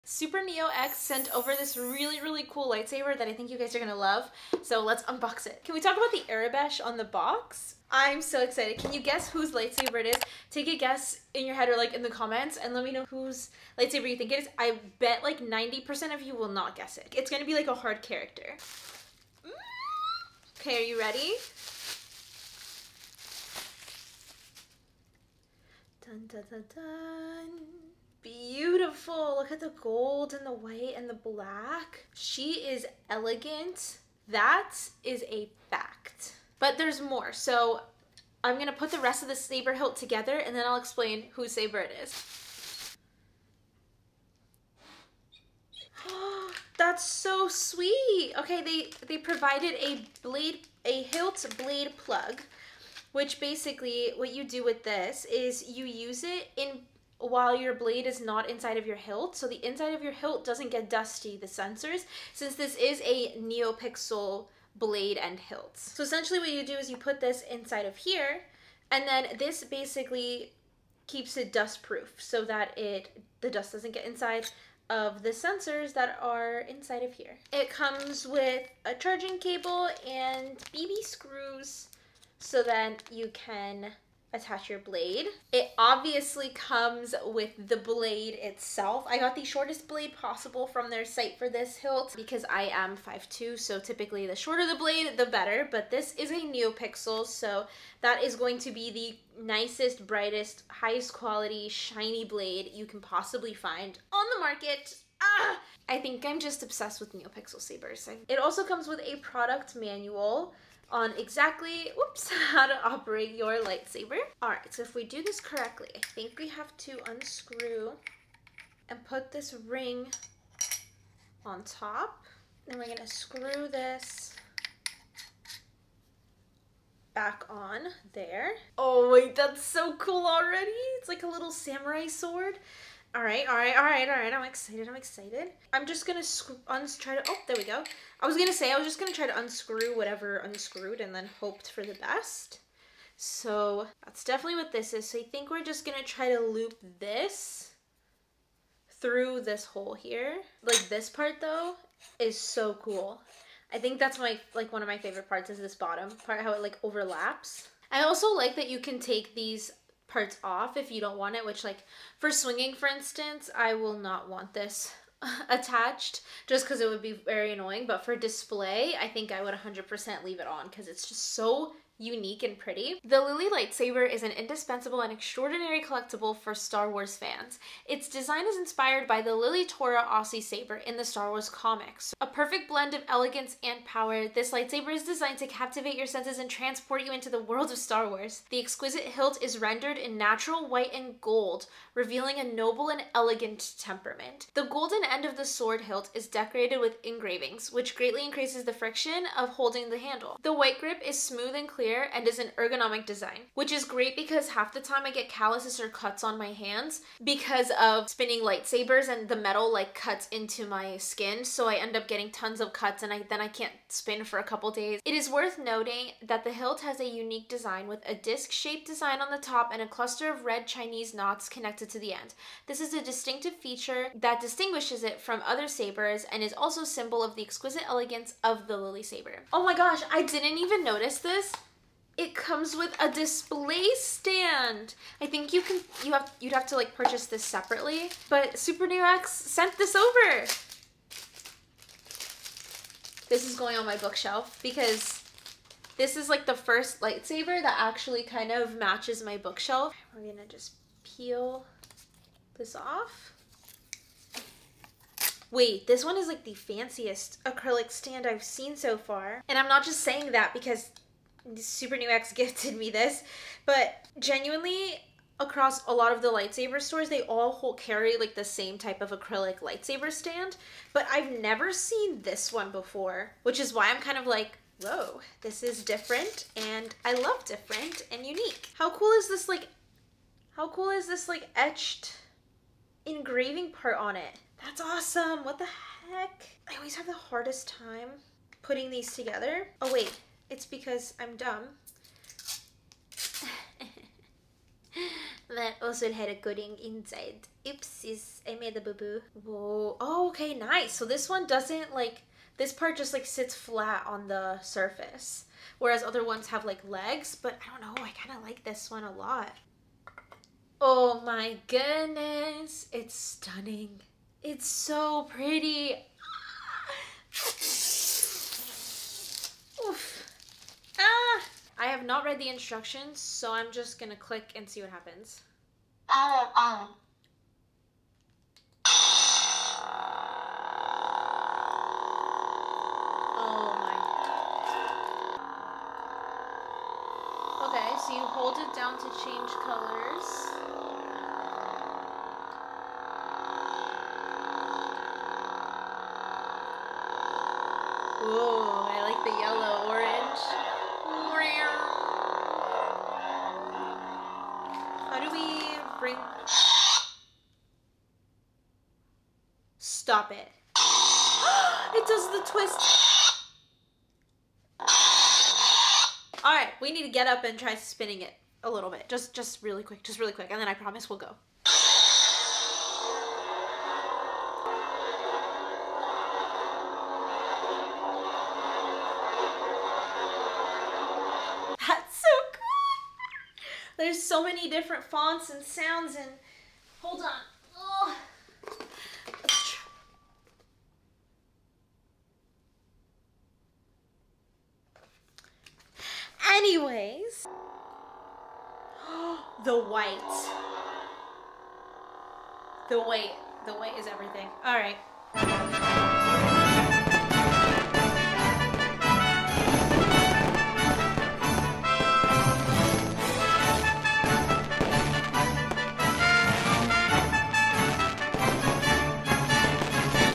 NEW Neopixel Lightsaber - Lily Unboxing from SUPERNEOX!
In today's video, Superneox was kind enough to send over their Lily lightsaber!